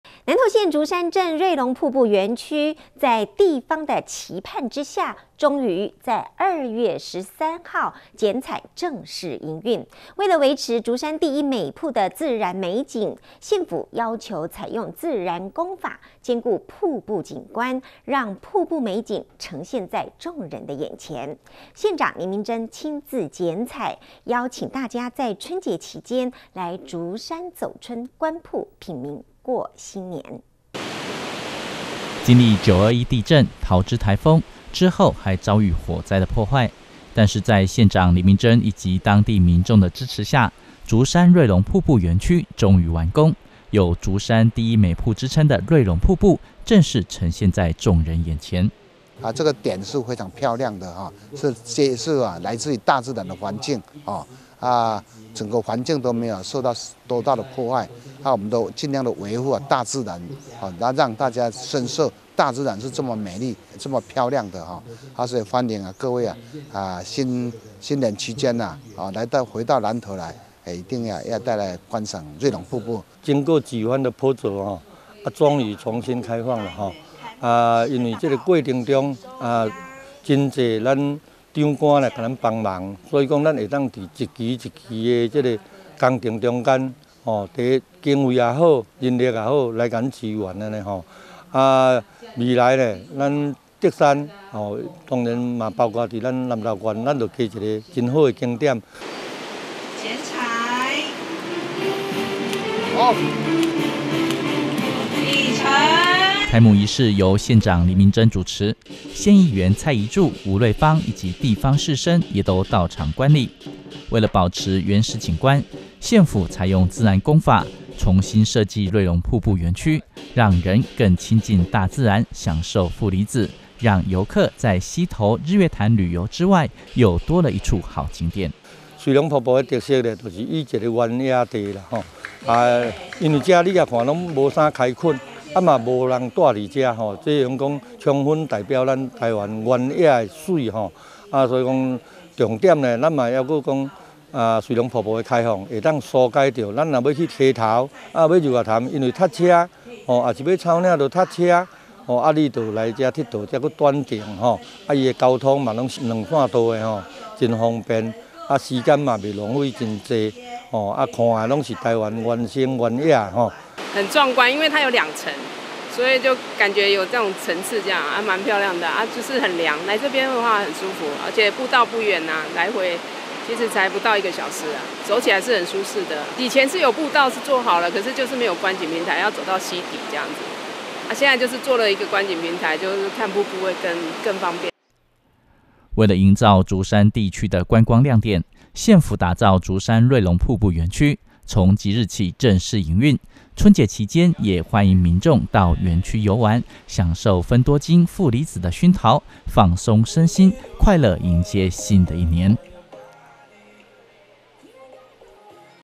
瑞龍瀑布重新開幕 營造竹山觀光新亮點
南投縣竹山鎮瑞龍瀑布園區，在地方的期盼下，終於在2月13號剪綵正式營運，為了維持「竹山第一美瀑」的自然美景，縣府要求採用自然工法兼顧瀑布景觀。讓瀑布美景呈現在眾人眼前。縣長林明溱親自剪綵邀請大家春節期間，來竹山走春，觀瀑、品茗過新年。